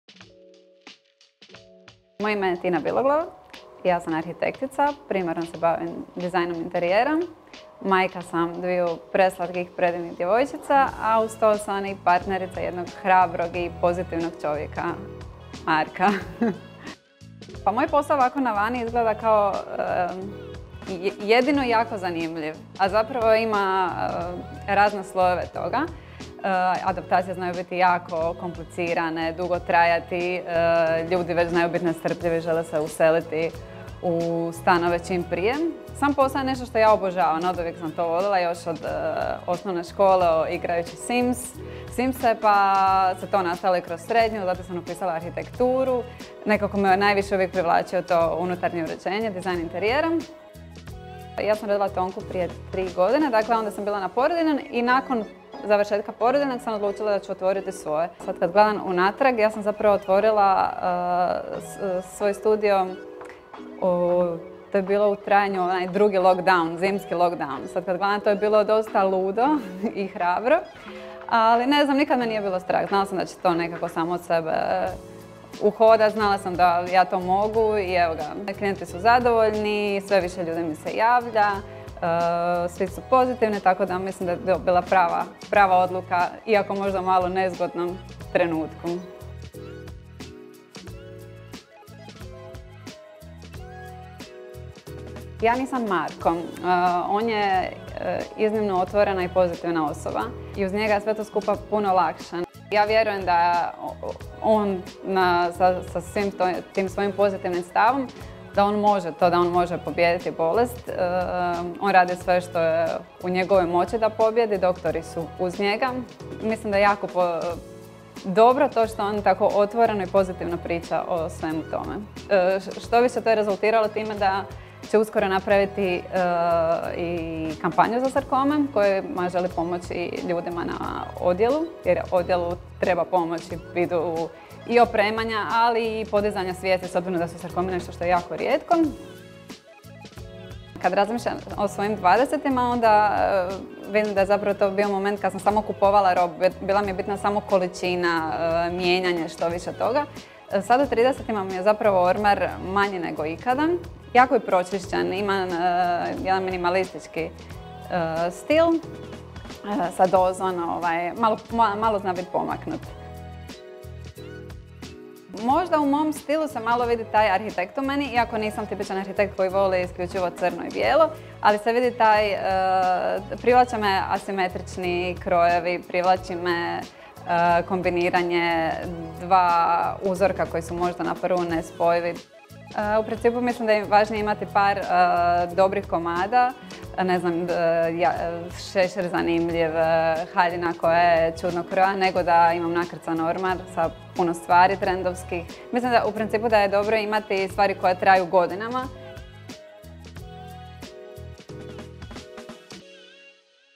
Arhitektica Tina Biloglav: 'Svoj studio otvorila sam tijekom lockdowna, bilo je to ludo i hrabro'
Tina je jedna od naših najperspektivnijih mladih arhitektica, majka je dvije djevojčice i partnerica Marka Babića, koji svojim pozitivnim stavom uveseljava društvene mreže i vrlo otvoreno govori o svojoj borbi sa sarkomom.